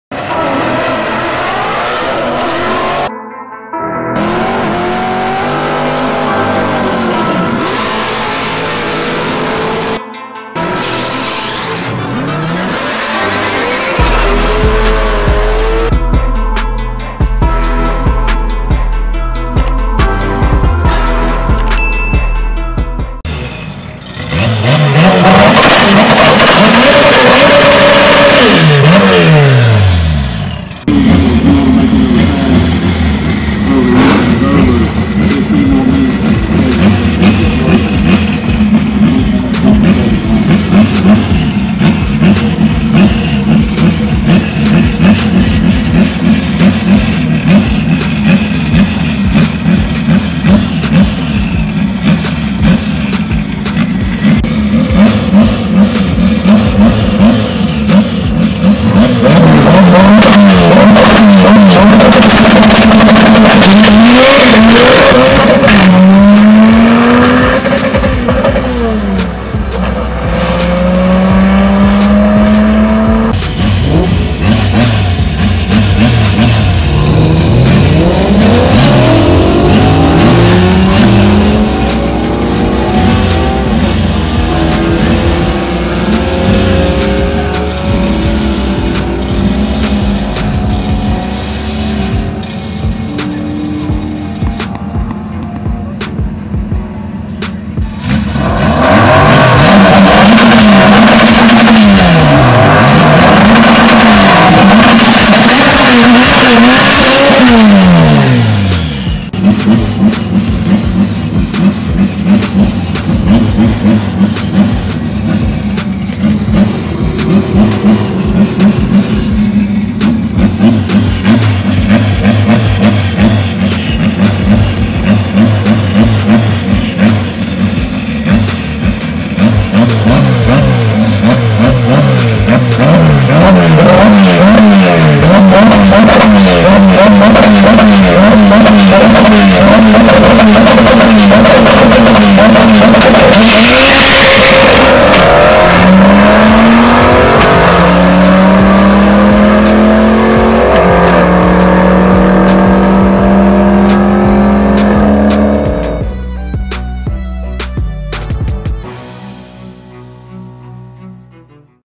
Turbo Acura Vs Dodge Challenger srt 392
This Turbo Acura didnt play is was crazy fast.